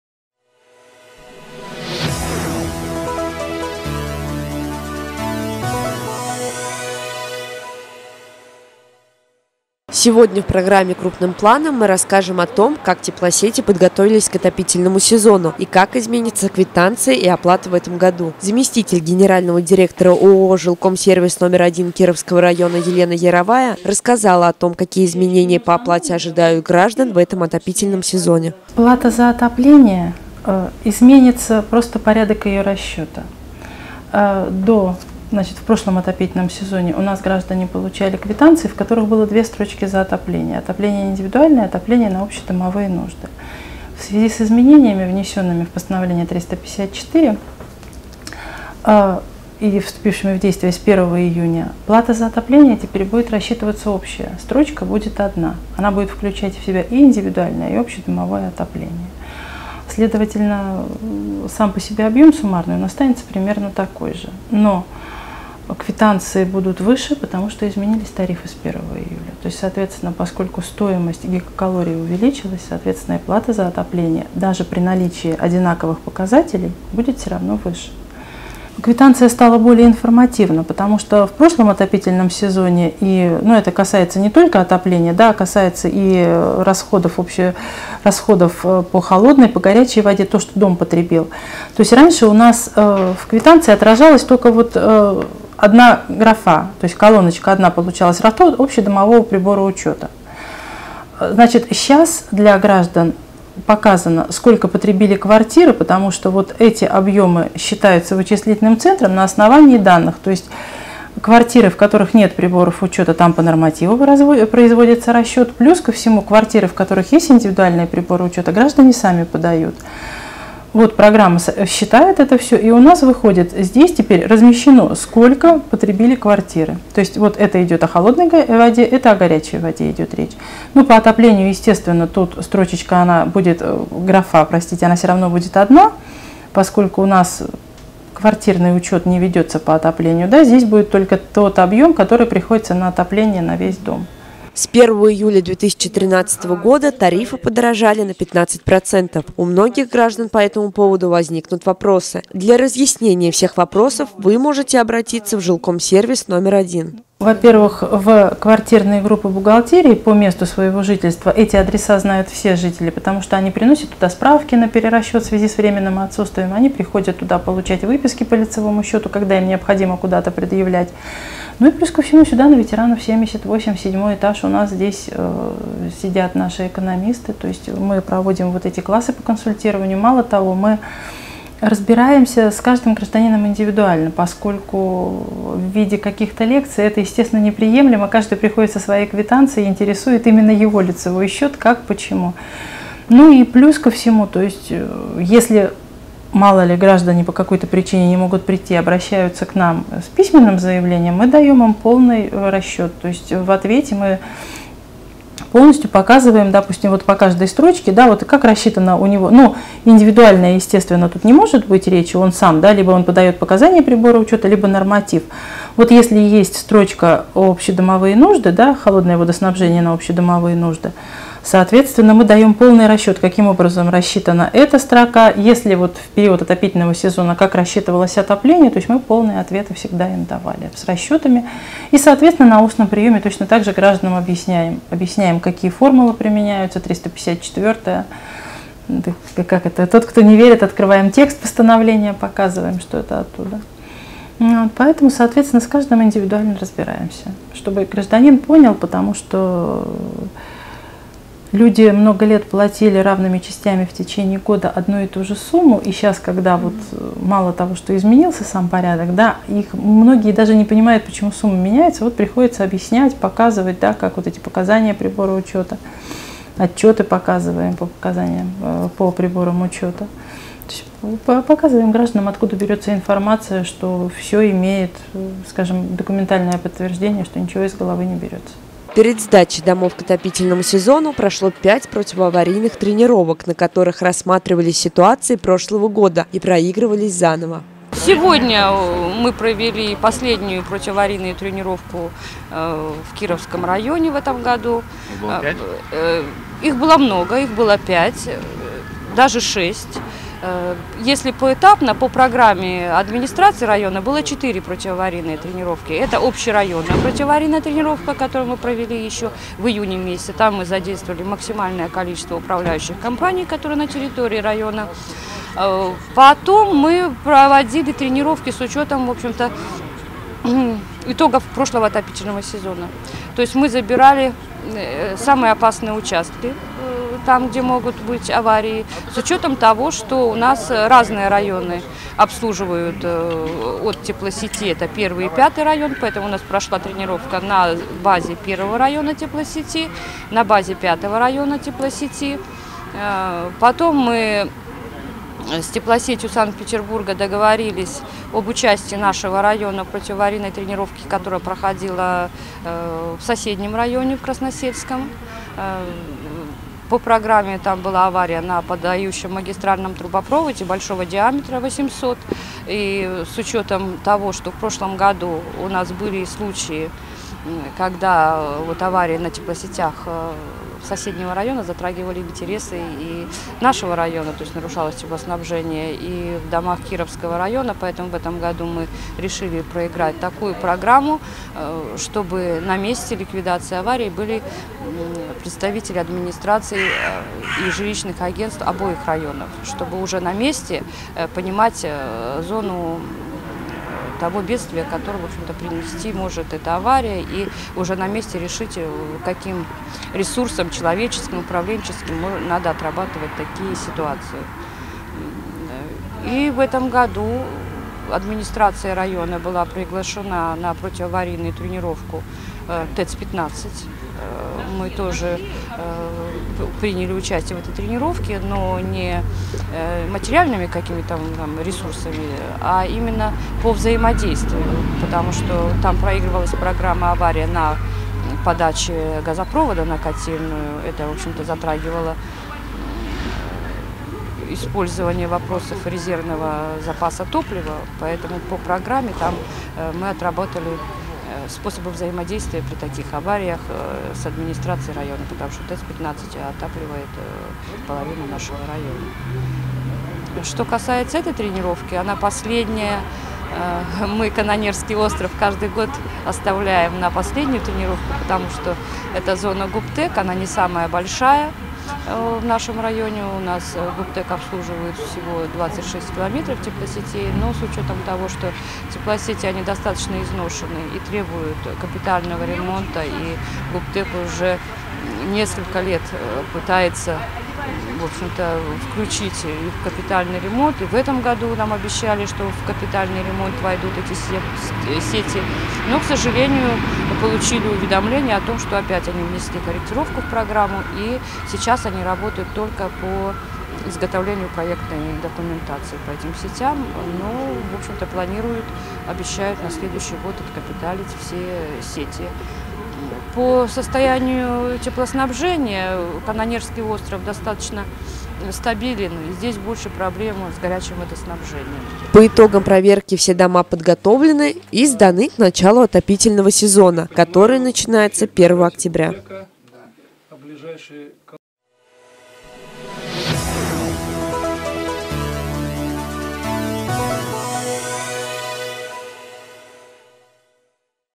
Новые тарифы ЖКХ вступают в силу
Противоаварийные тренировки прошли, теплосеть готова к новому отопительному сезону, новые тарифы по оплате